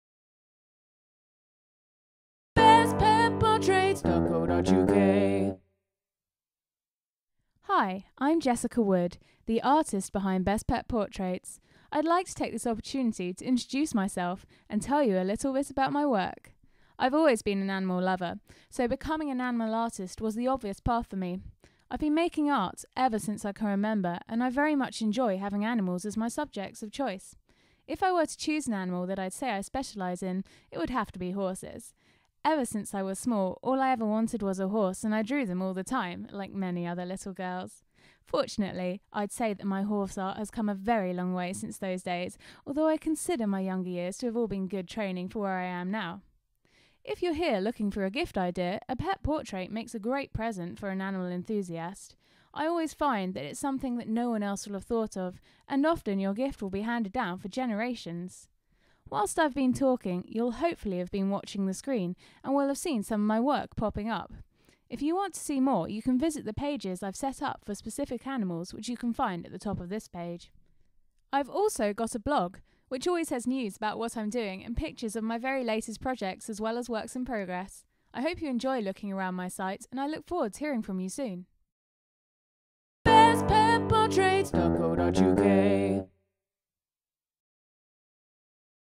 pet portrait artist uk, dog portrait artist
Jessica Wood is Best Pet Portrait's resident artist. Here she talks about her work, what inspires her and how you can find out more. Have a look at some of pet art which is displayed during this video. No pugs were harmed in the making of this!